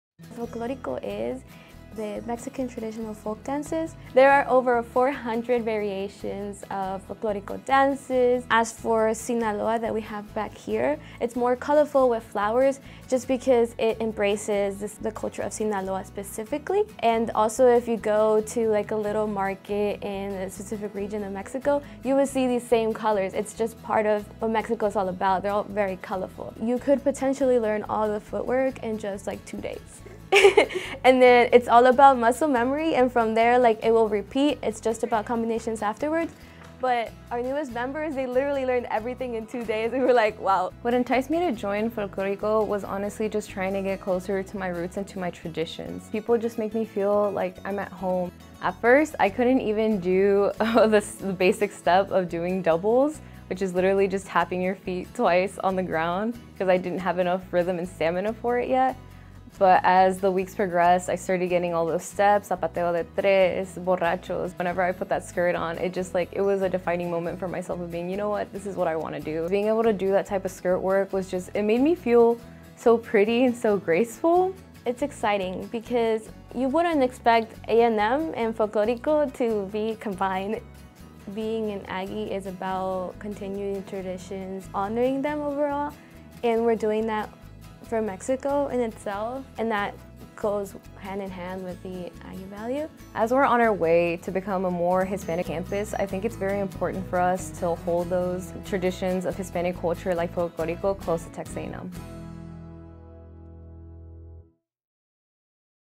Celebrating Mexican Culture with Ballet Folklorico
Members of Texas A&M Ballet Folklorico Celestial share stories of Hispanic history and culture through performing traditional Mexican folk dances on campus and in the community.